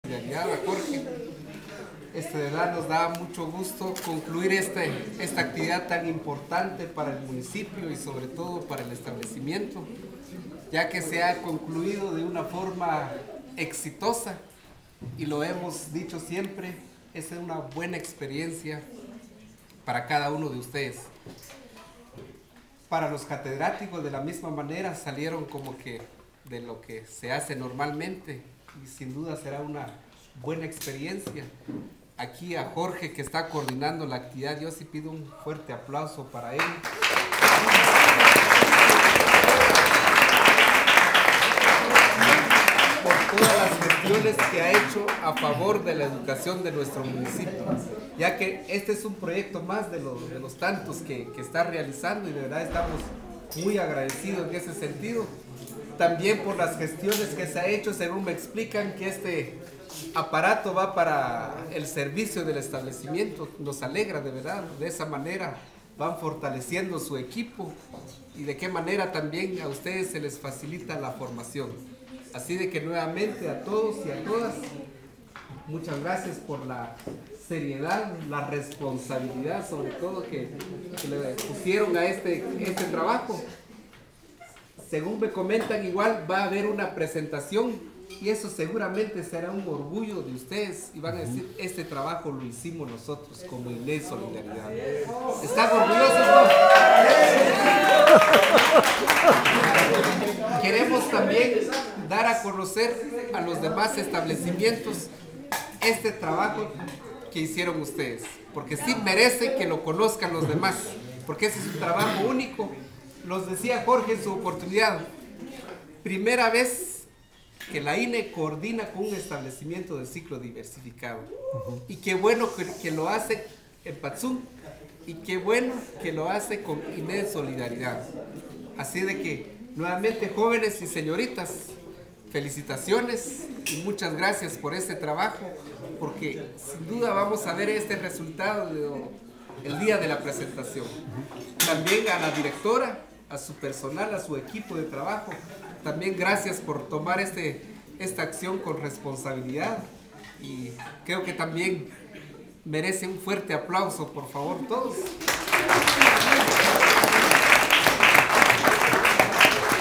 Palabras Abraham Batzibal
Patzún, Palabras Abraham Batzibal en el almuerzo de agradecimiento al Instituto INED Solidaridad por el trabajo realizado en la Encuesta EUTIC2014.